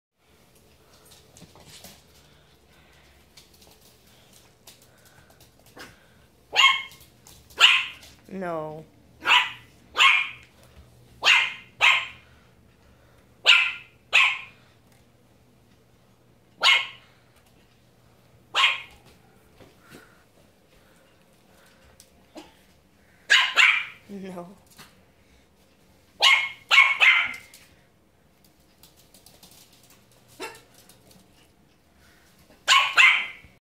Pomeranian puppy barking at broom!
Super funny. She barks at the broom.